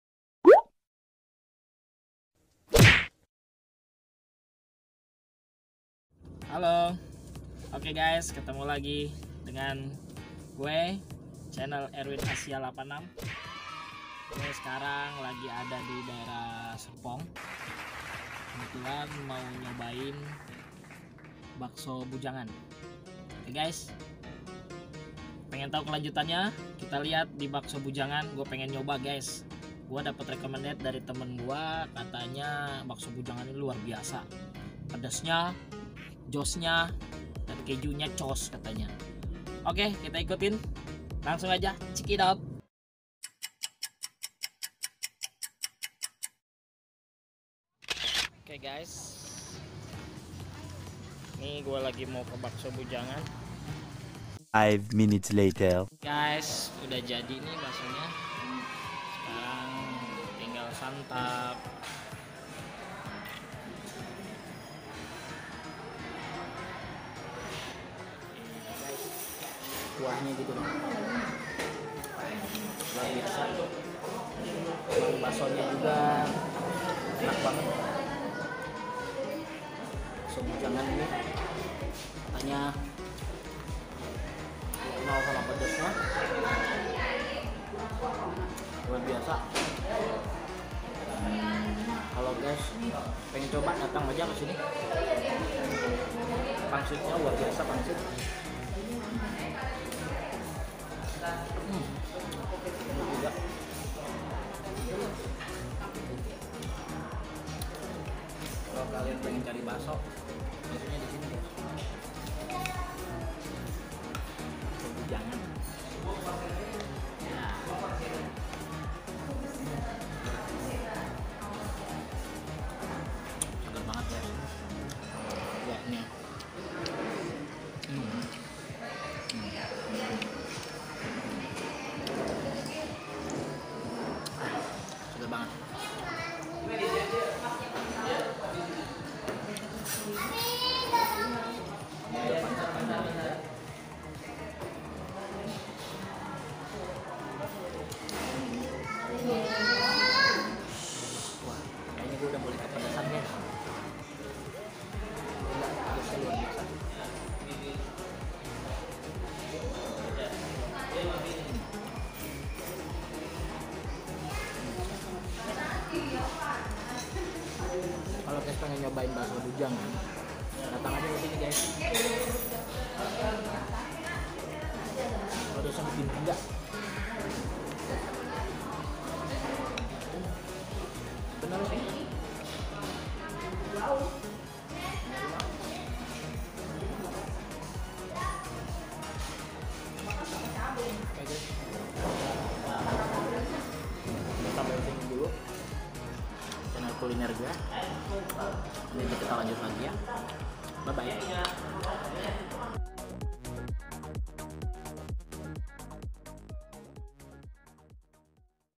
COBAIN BAKSO BOEDJANGAN|| Vlog-1😋||BERSAMA ERWIN ASIA 86||REKOMENDED BANGET GUYS👌🏻👍🏻
Assalamualaikum wr.wb

Ok guys di video kali ini gw mau cobain kuliner Bakso tapi bakso ini unik dari namanya yang beralamatkan di serpong tangerang (BAKSO BOEDJANGAN)yaitu kuliner yang lagi viral katanya bikin penasaran yang namanya aja bikin penasaran  bakso boejangan gw dpt  recommended dari teman gw ternyata baksonya setelah di icip² luar biasa enak guys pantas saja ramai pembeli dan pengunjung dari berbagai wilayah

Ok guys kali ini gw mau cobain bakso yg kuahya super pedas dan tekstur baksonya yg kenyal cukup padat juga dan rasanya beda dari bakso² lain yg pernah gue makan

Daftar menu  bakso boejangan sbb:
Kalian bisa klik link ini untuk daftar menu baso

Wassalamualikum wr.wb